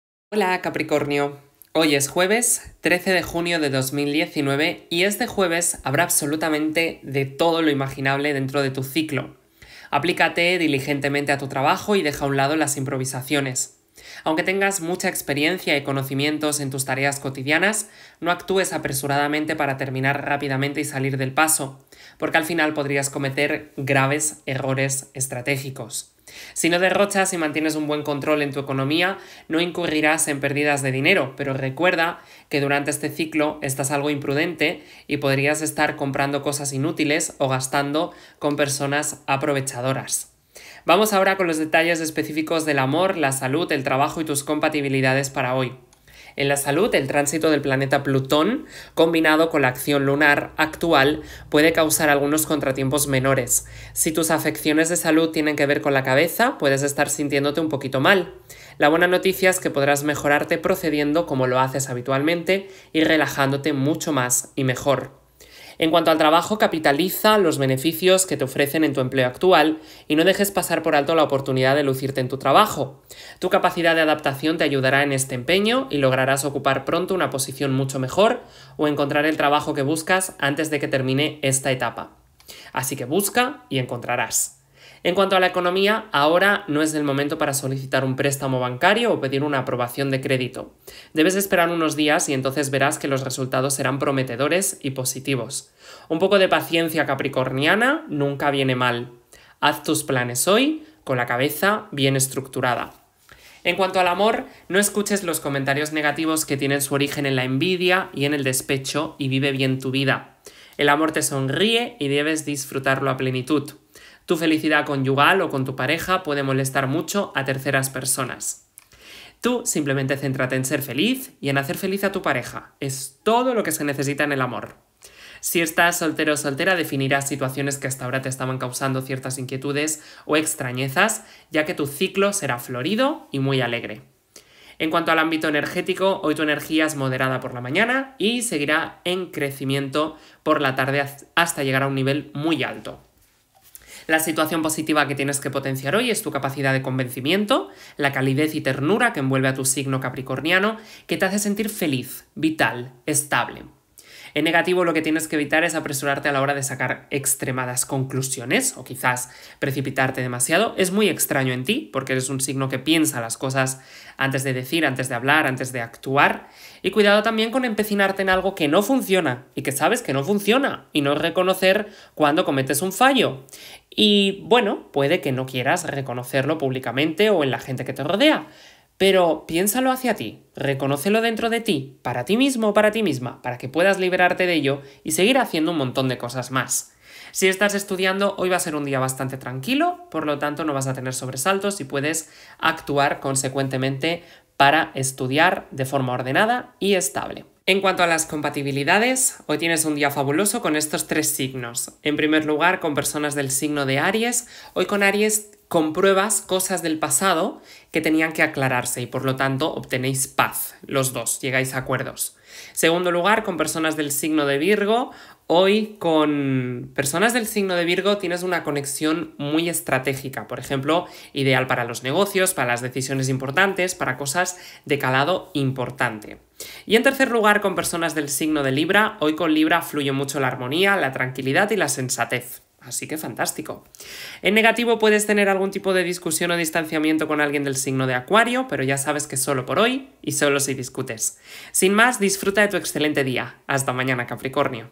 Horóscopo Diario - Capricornio - 13 de Junio de 2019
Horóscopo de hoy capricornio 13 de Junio de 2019. Horóscopo de capricornio hoy. Descubre tu futuro con mi horóscopo diario para ti, mi querido signo capricornio en el amor, el trabajo, la salud, el dinero y las compatibilidades con otros signos. Horóscopo diario gratis!